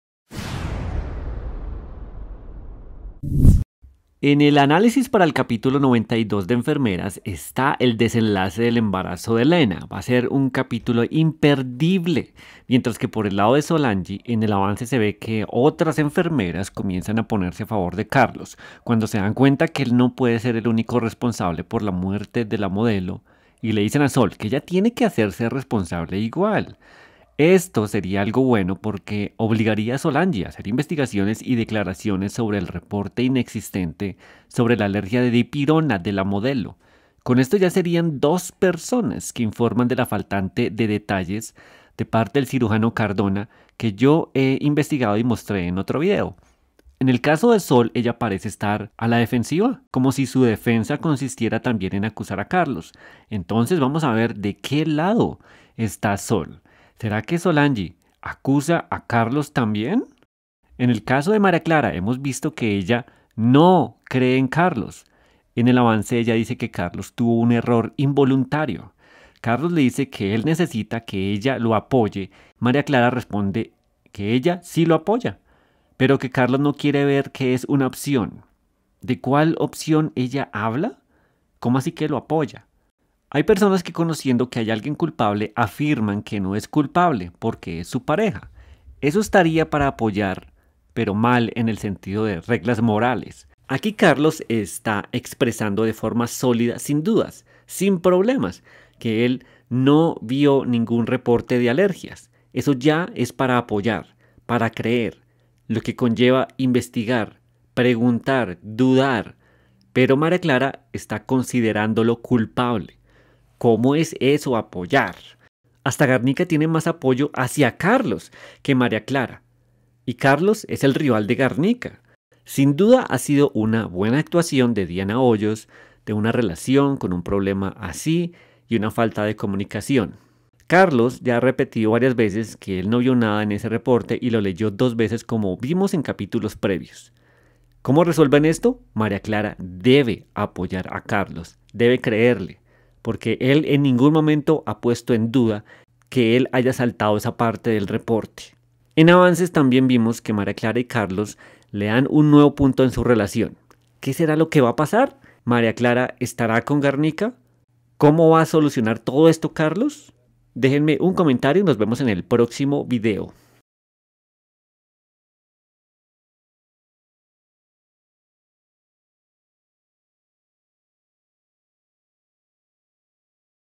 Enfermeras RCN, María Clara NO Cree A Carlos INOCENTE, T2 Capítulo 92, 25 Mayo 2021, ANÁLISIS
Enfermeras RCN, María Clara NO Cree A Carlos INOCENTE, T2 Capítulo 92, 25 Mayo 2021, ANÁLISIS del avance y predicciones